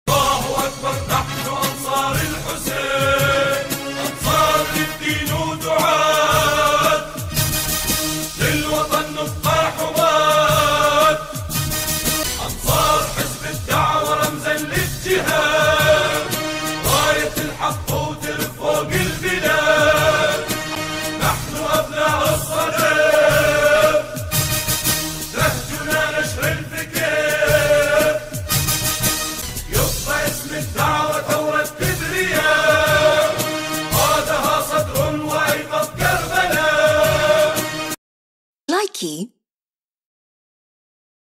بيان حزب_الدعوة_الإسلامية:بخصوص منح الاردن إجازة لحـ.زب البعـ.ث المقبـ.ور 👇الوصف مهم
فوجئ العراقيون وانتابتهم الصـ.دمة والغضـ.ب من خبر منح الاردن إجازة لحـ.زب البعـ.ث المقبـ.ور بممارسة نشاطه السياسي ويعد ذلك عملا عـ.دائيا وأستفـ.زازيا.